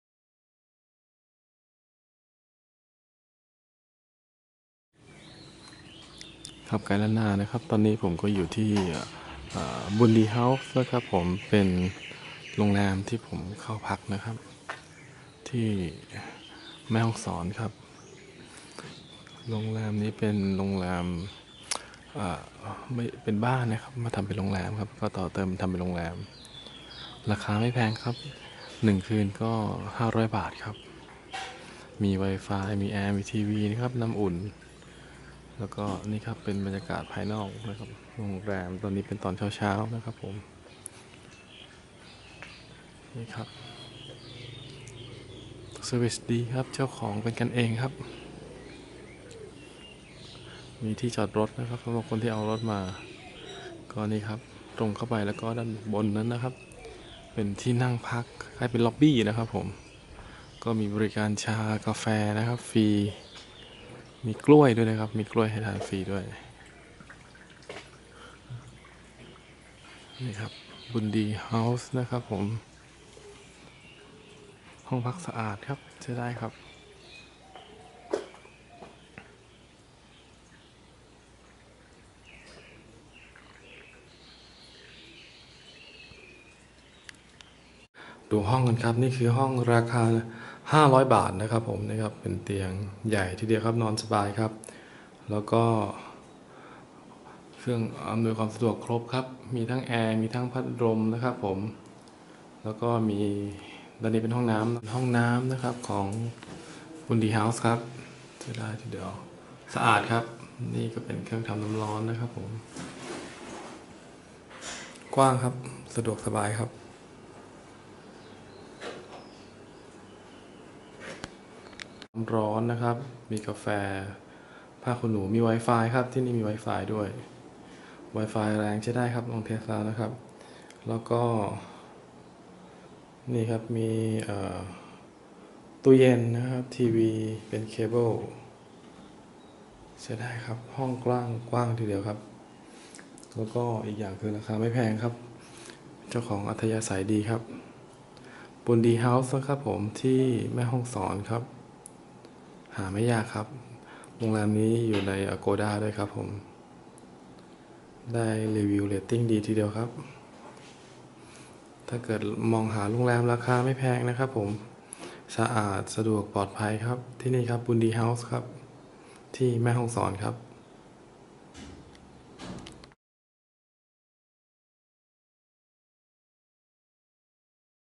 บุญดีเฮ้าส์ ห้องพักที่แม่ฮ่องสอน Boondee hosue Mae Hong Son
การเดิoท่องเที่ยวแม่ฮ่องสอนหน้าแล้งของผมครั้งนี้ ผมเลือกพักที่บุญดีเฮ้าส์ เป็นห้องพักที่ อยู่ใจกลางเมืองแม่ฮ่องสอน สะอาด สะดวกปลอดภัย มีที่จอดรถ ไม่ต้องใช้รถขับไปใหนอีกแล้ว จอดไว้แล้วเดินเที่ยวหาร้านอาหาร ทั้งตอนเช้าและตอนเย็นที่ในเมืองแม่ฮ่องสอนใด้เลย